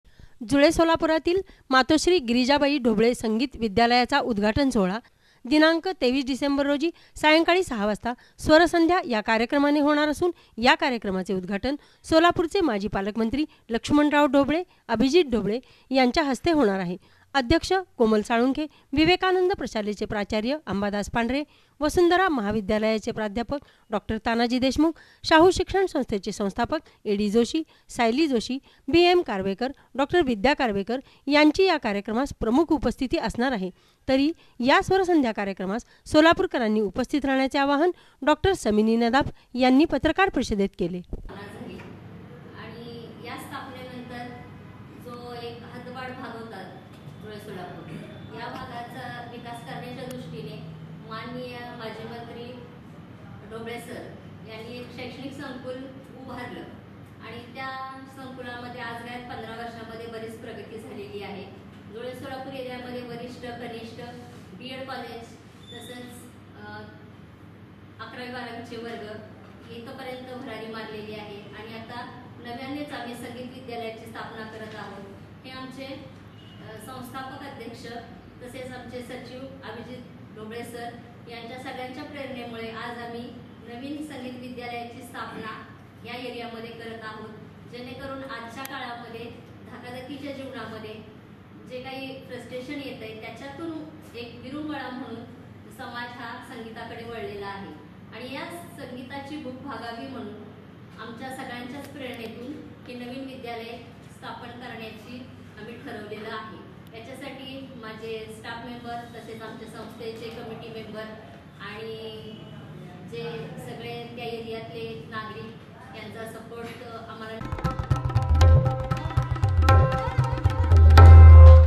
23 डिसेंबरला ढोबळे संगीत विद्यालयाचे उद्घाटन
सोलापूर : जुळे सोलापुरातील मातोश्री गिरीजाबाई ढोबळे संगीत विद्यालयाचा उद्घाटन सोहळा दि.23 डिसेंबर 2018 रोजी सायंकाळी 6 वाजता स्वरसंध्या या कार्यक्रमाने होणार असून या कार्यक्रमाचे उद्घाटन सोलापुरचे माजी पालकमंत्री लक्ष्मणराव ढोबळे, अभिजीत ढोबळे यांच्या हस्ते होणार आहे.
अध्यक्ष कोमल साळुंखे, विवेकानंद प्रशालेचे प्राचार्य अंबादास पांढरे, वसुंधरा महाविद्यालयाचे प्रा.डॉ.तानाजी देशमुख, शाहू शिक्षण संस्थेचे संस्थापक ए.डी.जोशी, सायली जोशी, बी.एम.कार्वेकर, डॉ.विद्या कार्वेकर यांची या कार्यक्रमास प्रमुख उपस्थिती असणार आहे.
तरी या स्वरसंध्या कार्यक्रमास सोलापुरकरांनी उपस्थित राहण्याचे आवाहन डॉ.समिनी नदाफ यांनी पत्रकार परिषदेत केले.